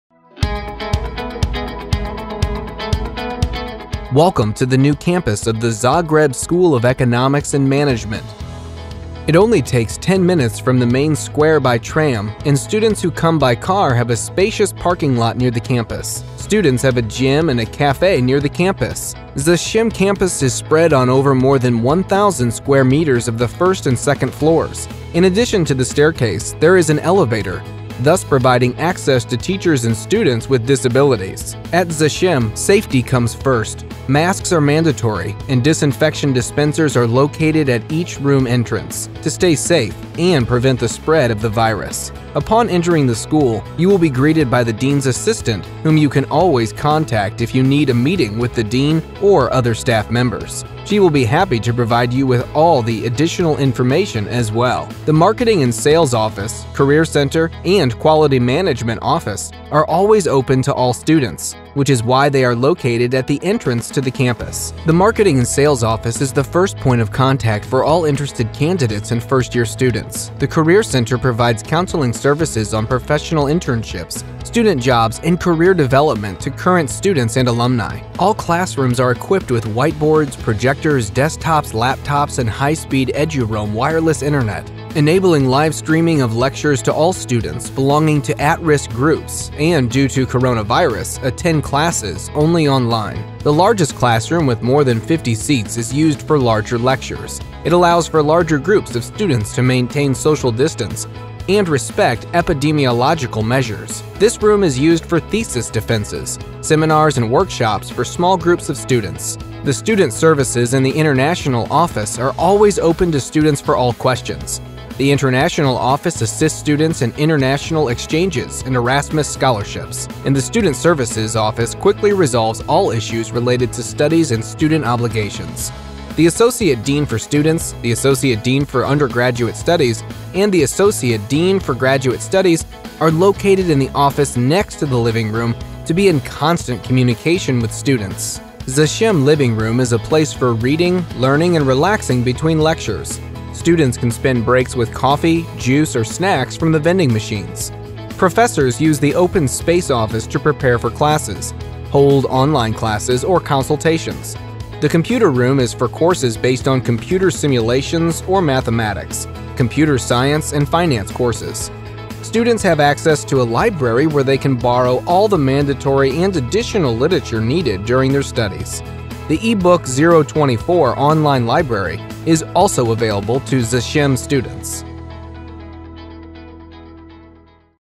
New ZSEM campus
ZSEM moved to a new, modern campus in Vukasovićeva street 1.